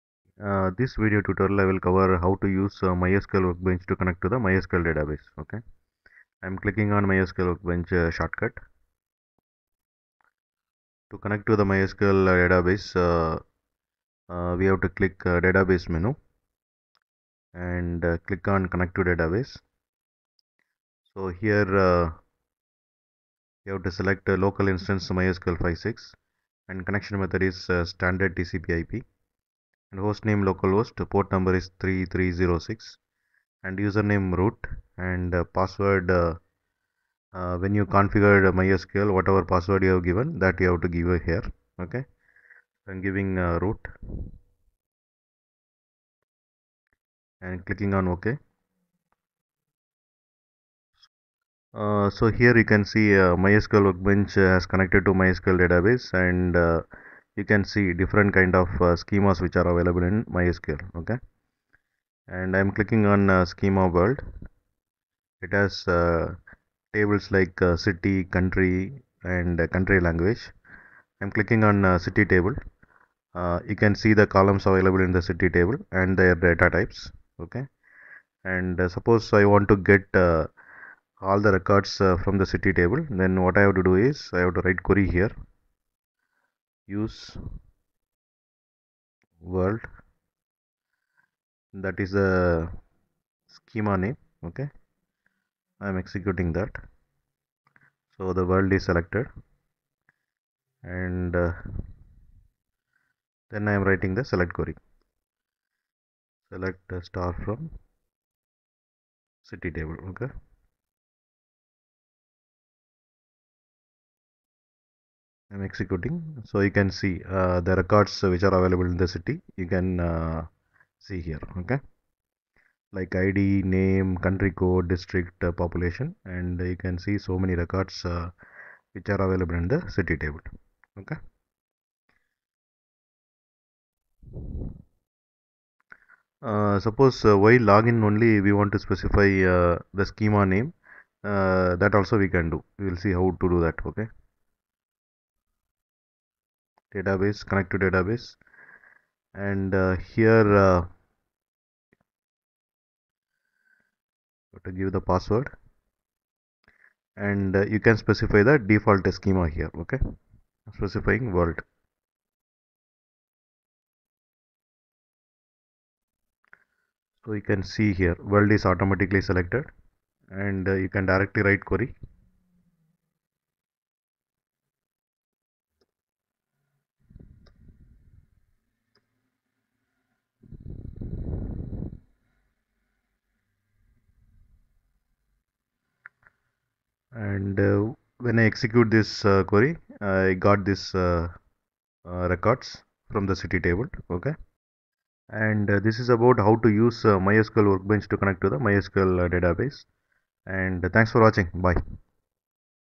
MySQL Workbench Tutorial: How to Connect to MySQL Database
In this step-by-step tutorial, you'll learn how to connect MySQL Workbench to your MySQL database. MySQL Workbench is a powerful tool that allows you to manage MySQL databases efficiently with its user-friendly interface. Whether you're a beginner or an experienced developer, mastering this connection process is essential for effective database management.

In this video, we'll cover the following topics:
- Installing MySQL Workbench: We'll guide you through the installation process to ensure you have MySQL Workbench set up on your system.
- Configuring MySQL Connection: Learn how to configure MySQL Workbench to establish a connection with your MySQL database server.
- Connecting to MySQL Database: Follow along as we demonstrate the step-by-step process to connect MySQL Workbench to your MySQL database.
- Executing Queries: Once connected, we'll show you how to execute SQL queries directly from MySQL Workbench to interact with your database.

By the end of this tutorial, you'll have a solid understanding of how to use MySQL Workbench to connect to your MySQL database, empowering you to manage your data more effectively.